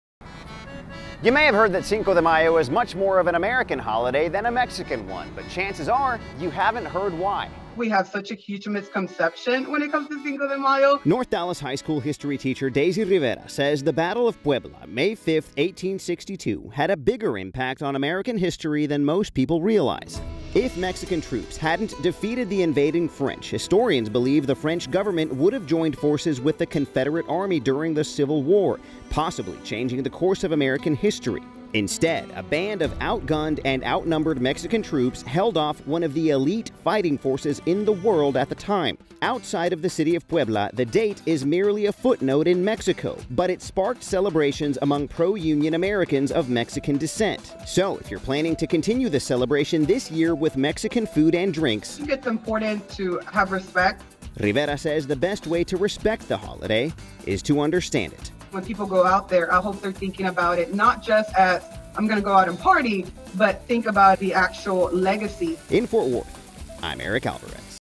How the Battle of Puebla impacted American history, sparking the holiday Cinco de Mayo
If Mexican troops hadn't held off the invading French, historians believe the French gov't would have joined forces with the Confederate Army during the Civil War.